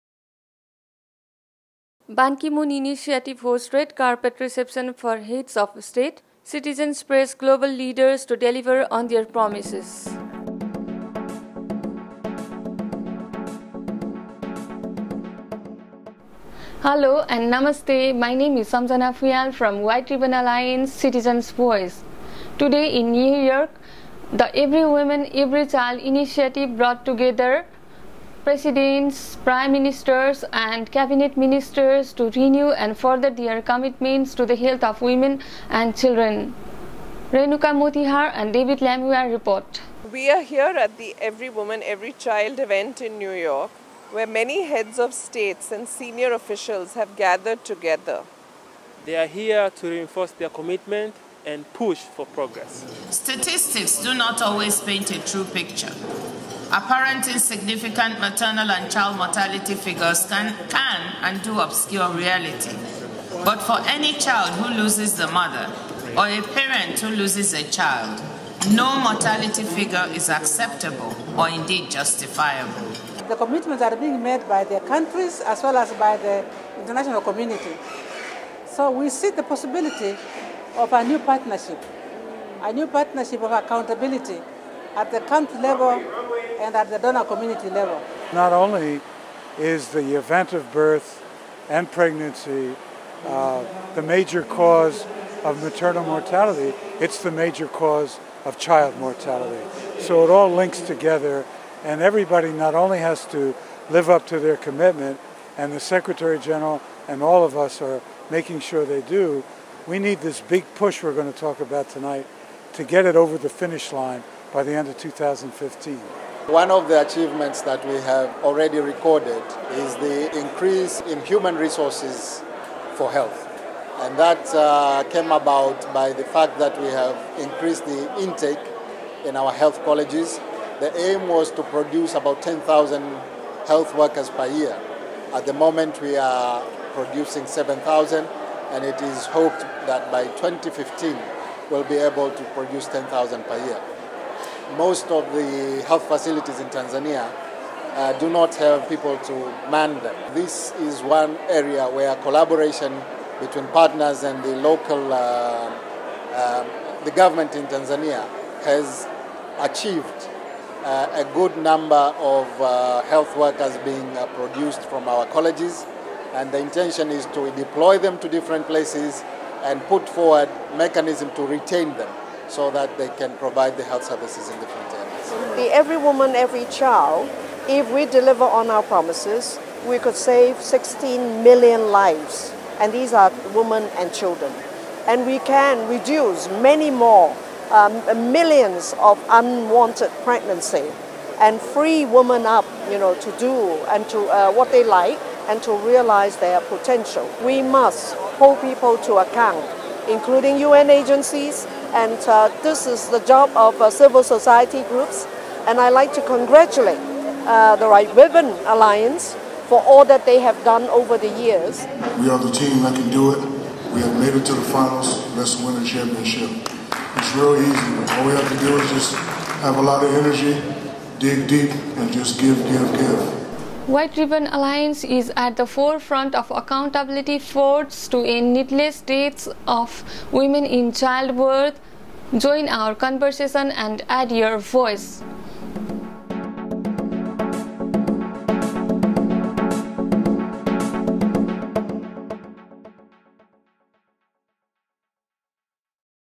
Citizens' Voice: The Big Push for Every Woman and Every Child
The White Ribbon Alliance Citizens' Voice team are reporting from the UN General Assembly, amplifying voices of our national leaders and demanding accountability.Here we report from the Every Woman Every Child Reception where Governments, civil society, UN organisations, members of the private sector and maternal health champions have gathered to celebrate progress, and commit to the ''big push''.